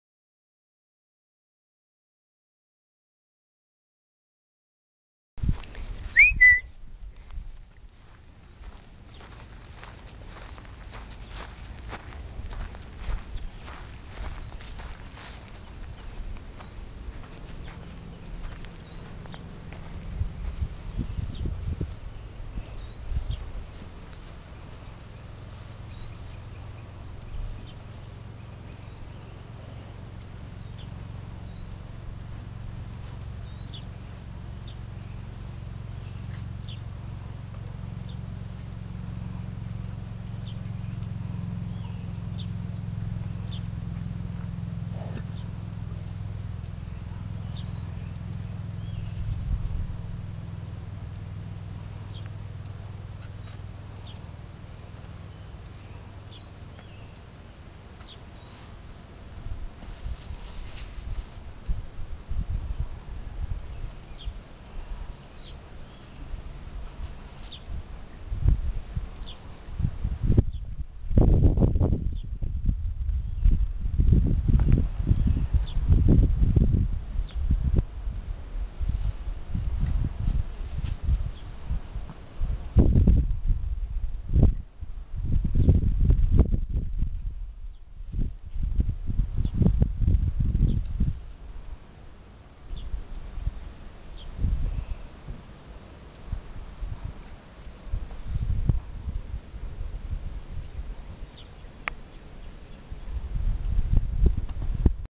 Cal and the plastic bag 7-9-10
Cal playing with a plastic bag